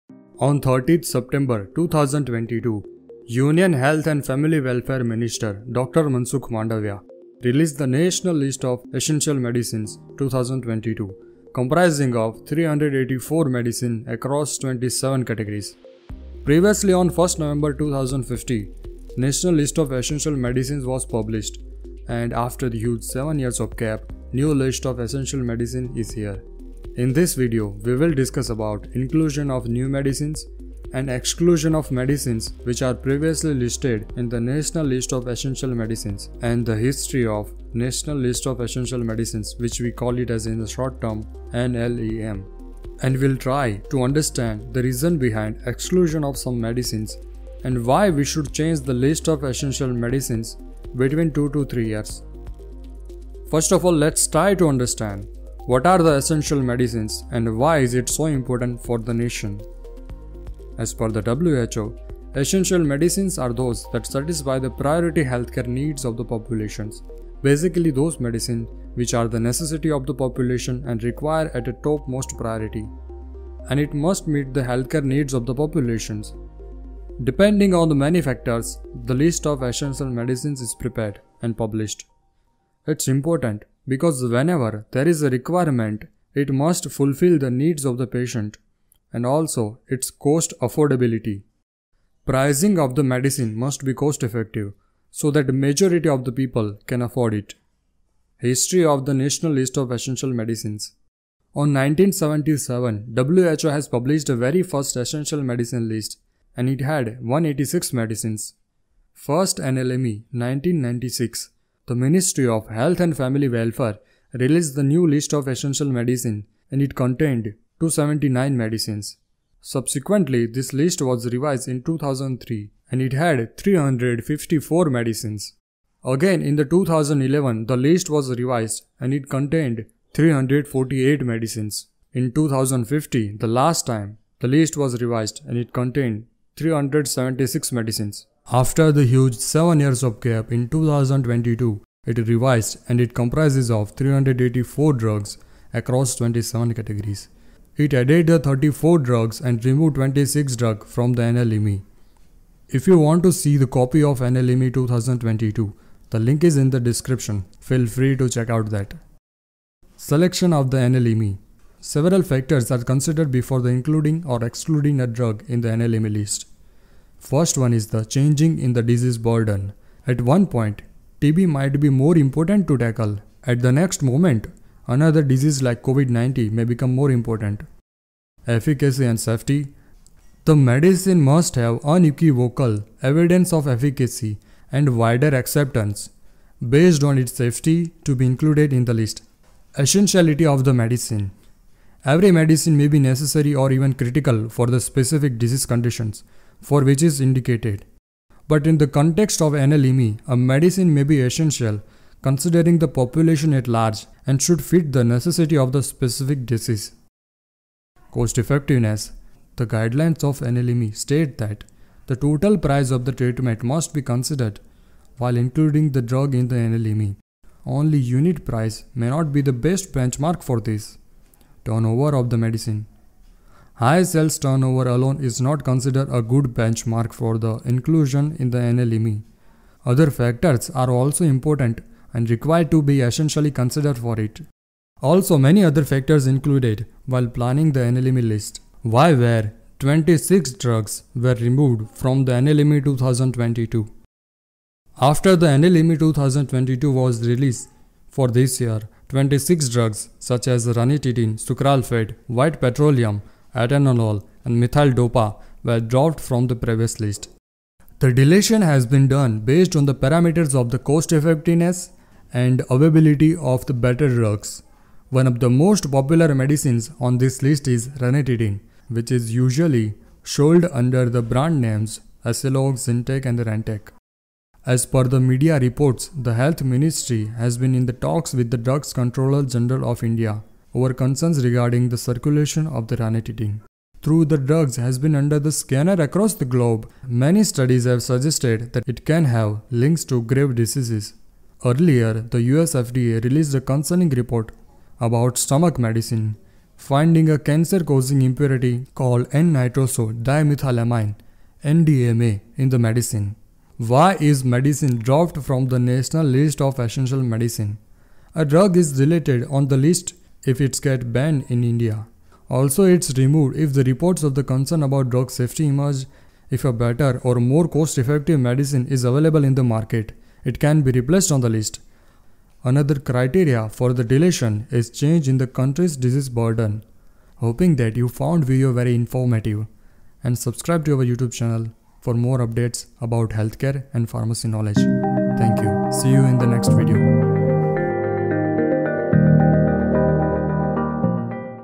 National List Of Essential Medicines | NLEM 2022 | Pharmaclysta
National List of essential medicine released in India on13the September 2022,
Here in this video, we will discuss everything that you need to know?
and Why it's important for the nation?

Equipment I Used For Video